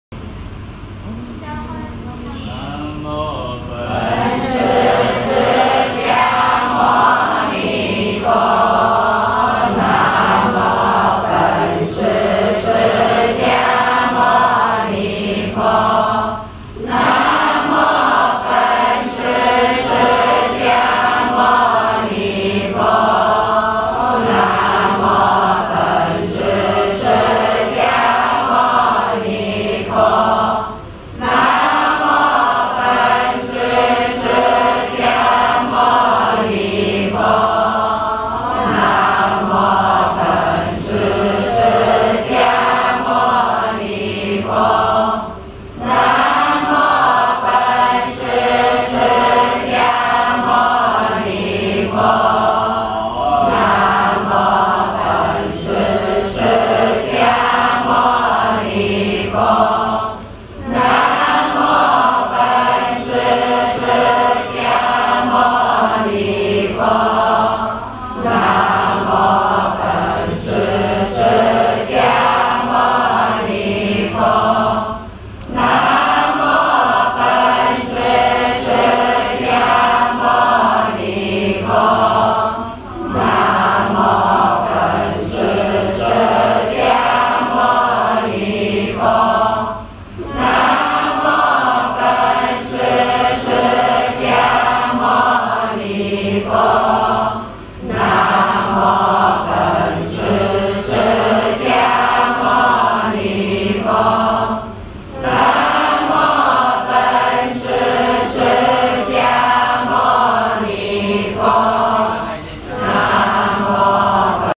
净慧禅寺 菩提伽耶～10
开祥大和尚领众念佛号供袈裟